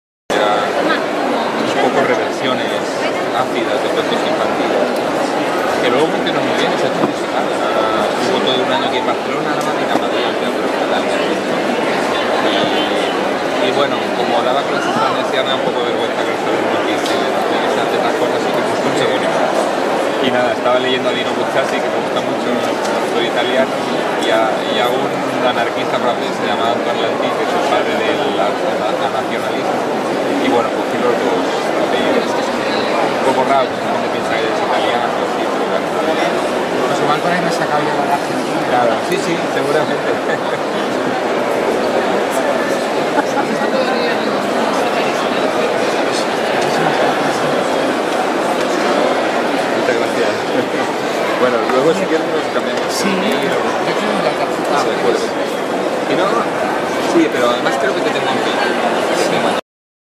Un minut de Nob
Nob dibuixant al 32è Saló del Còmic de Barcelona 2014.